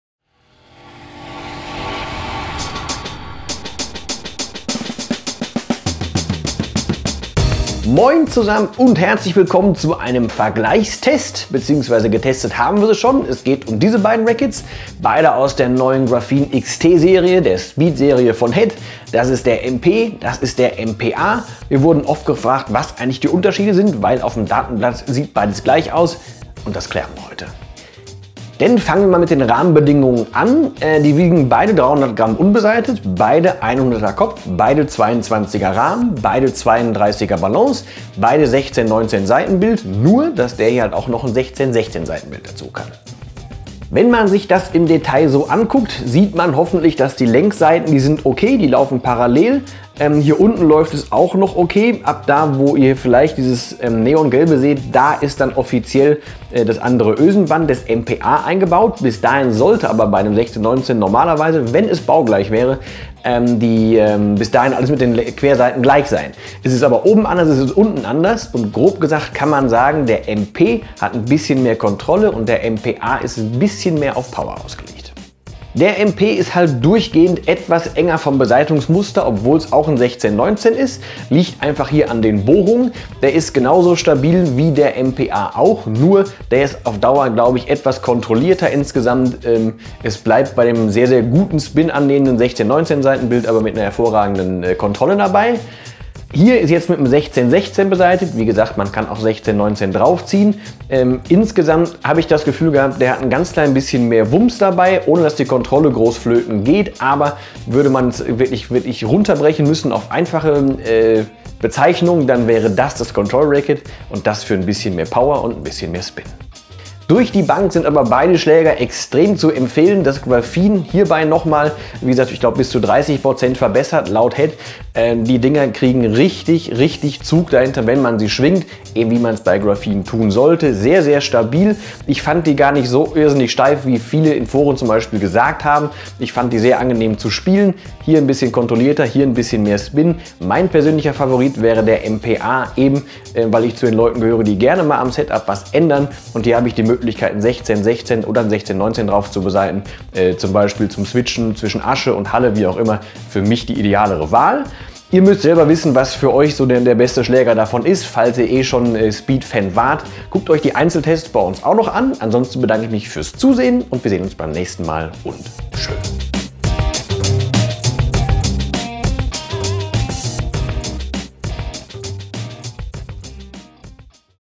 VLOG Vergleich Head Graphene XT Speed MP & MP A
Die neuen Head Graphene XT Speed Modelle sind da und bringen auch die Adaptive String Pattern mit sich. Viele haben sich gefragt: Was ist der Unterschied zwischen Speed MP und Speed MP A? Die Antwort gibt es in diesem Video :)